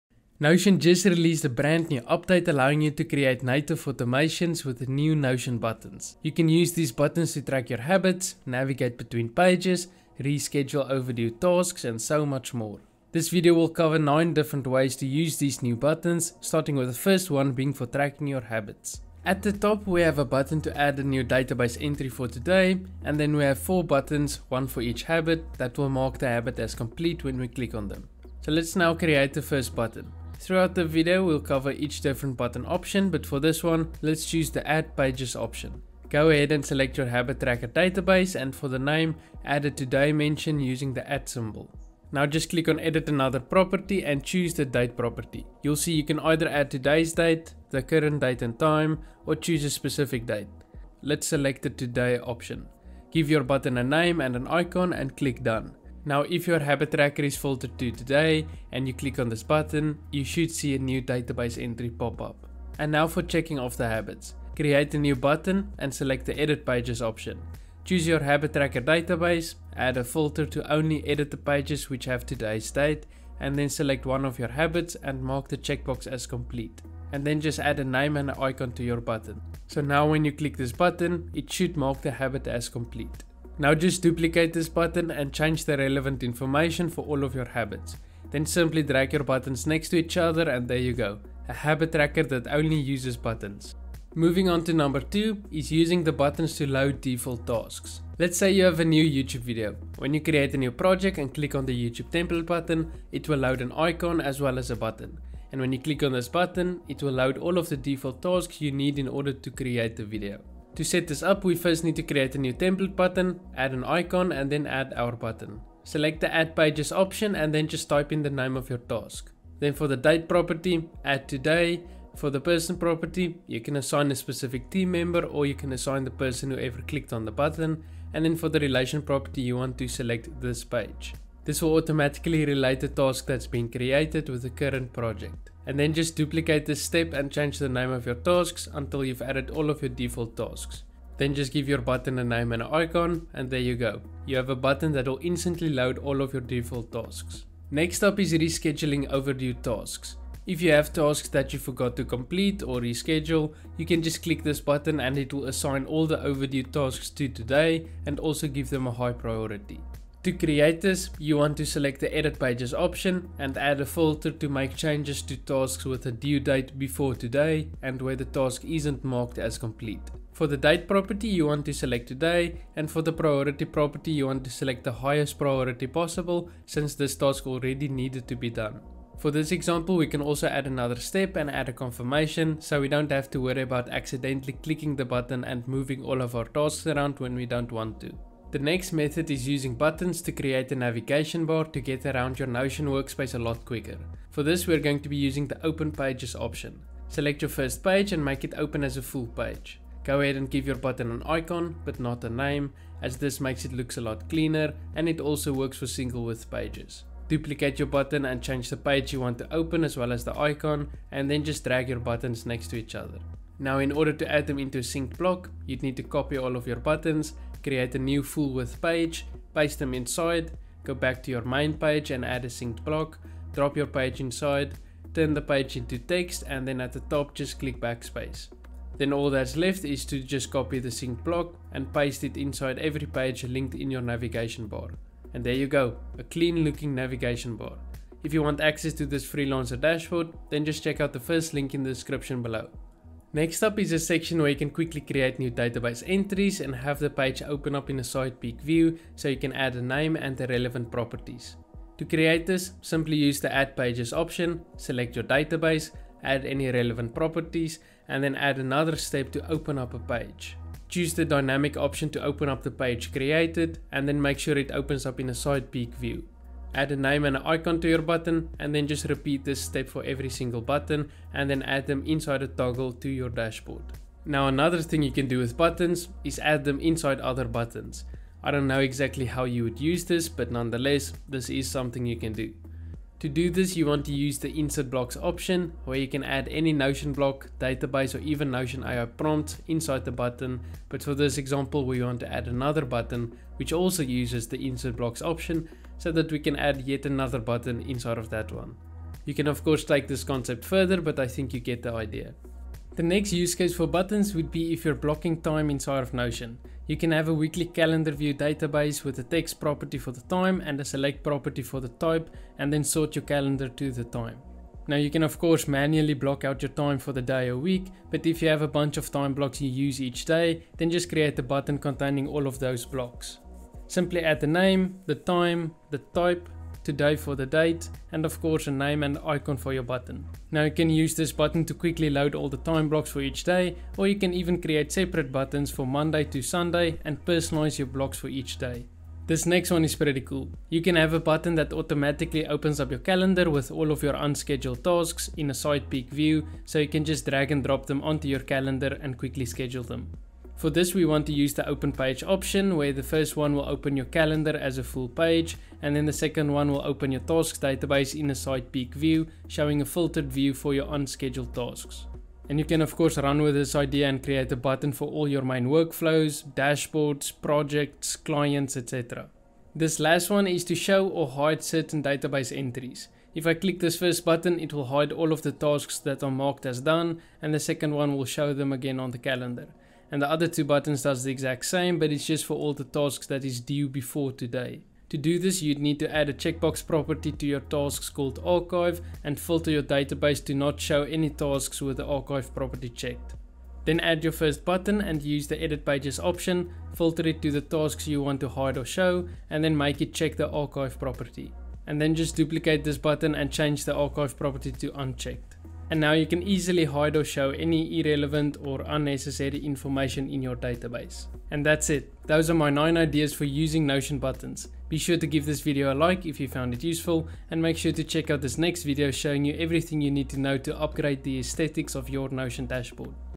9 Useful ways to use the NEW Notion buttons!
Native Automation in Notion! These new Notion buttons are absolutely amazing and super powerful, which is why in this video I'm covering 9 different ways you can use the new Notion buttons in your own Notion workspace.

Timestamps:
00:00 Intro
00:17 1. Tracking your habits
01:34 2. Loading default tasks
02:28 3. Rescheduling overdue tasks
03:08 4. Navigation bar
04:03 5. Quick create buttons
04:35 6. Buttons inside buttons
05:03 7. Time blocking
05:39 8. Side peek databases
06:10 9. Show/Hide Tasks

🎧 Background Music